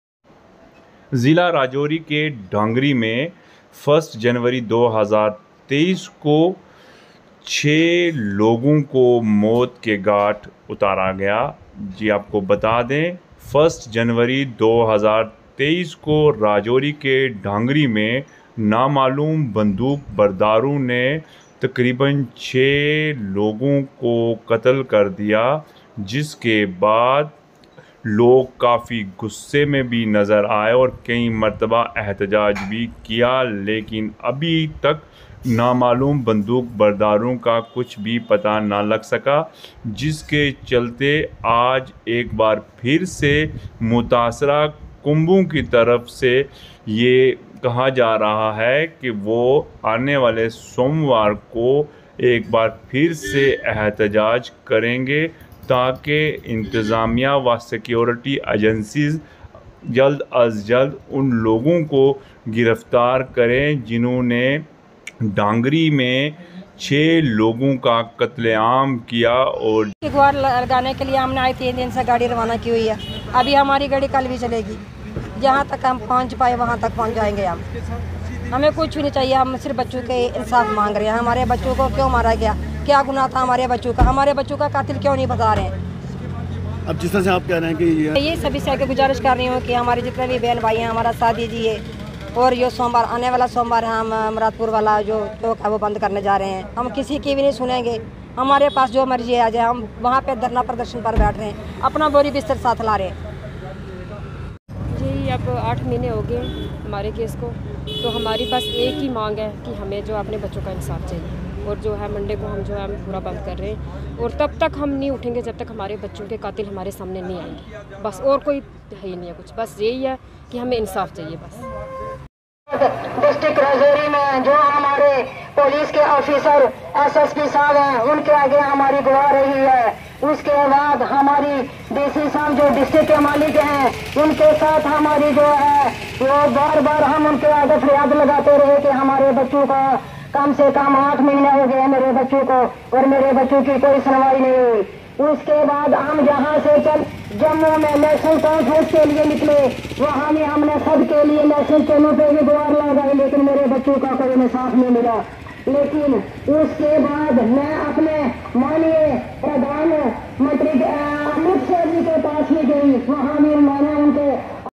The Dhangri victim families have once again announced a protest sit-in on monday.
On January 01, 2023, 6 innocent people were killed by unknown gunmen in Dhangri of district Rajouri. The victim families of Dhangri said why there is so much delay in justice...